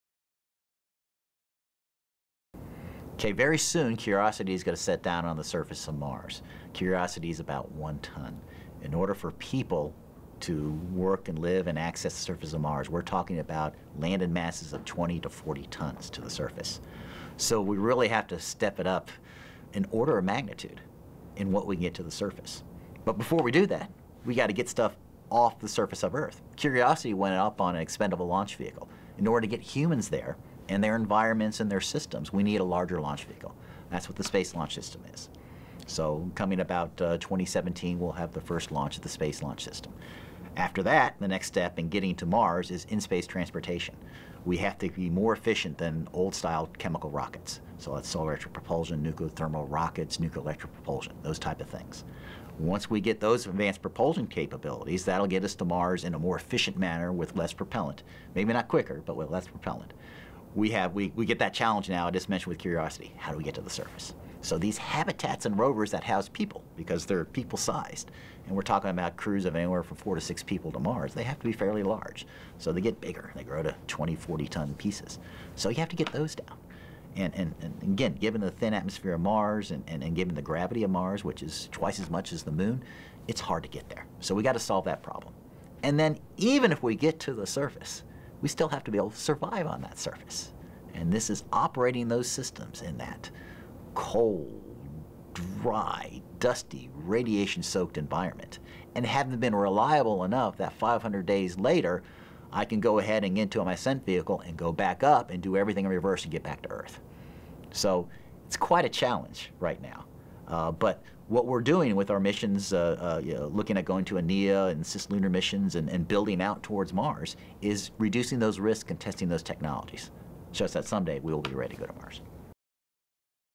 Pat Troutman on Challenges of Sending Humans To Mars
Pat Troutman, Human Exploration Strategic Analysis Lead, NASA Langley
Research Center, responds to questions from the public about sending
humans to Mars.